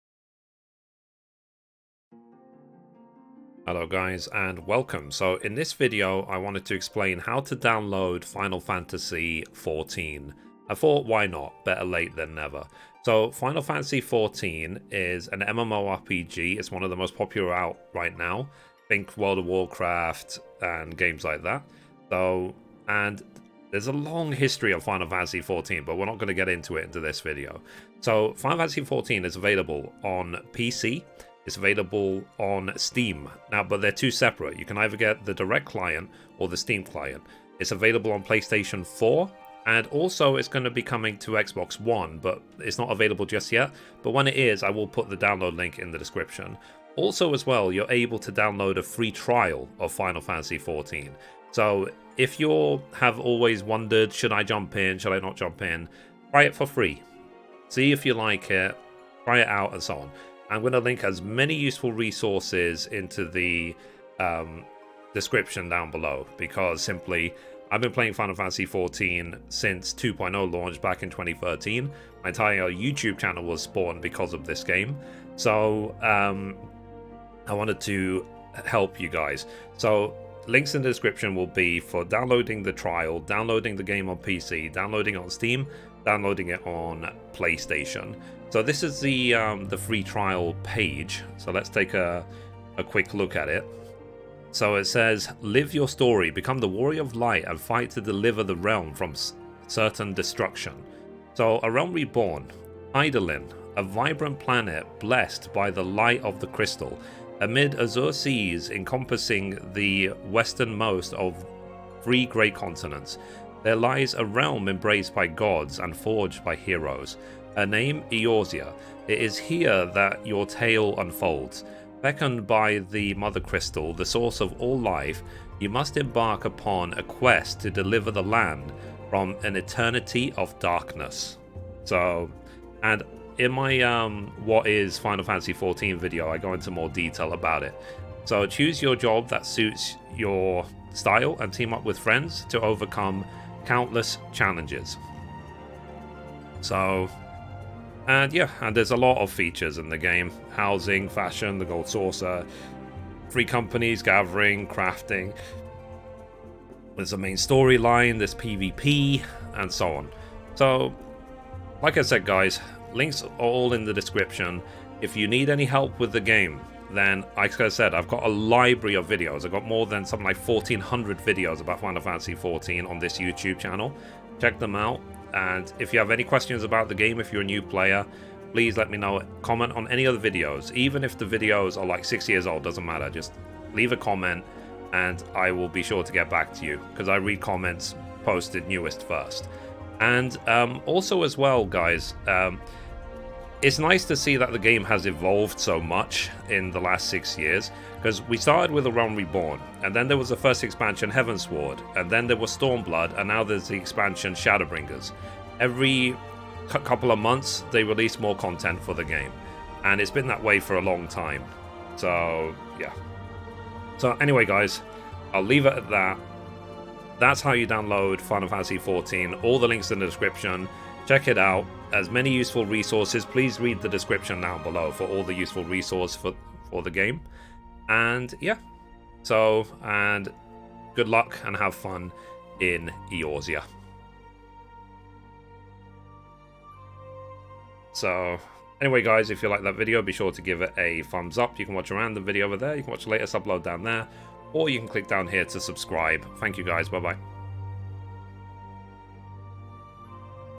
Final Fantasy XIV Download Tutorial Guide (Beginner)
In this video, I show you how and where to download Final Fantasy XIV otherwise known as FFXIV. The game is available on PC (via Square Enix Client or Steam), Mac, PS4, PS5 and Xbox.

Final Fantasy XIV is an MMORPG based in the Final Fantasy universe, combining tab-targetted action, an in-depth story, great music, crafting, gathering and many more features.

FFXIV was previously available on PS3 but the service for this version of the game was discontinued.

Mithrie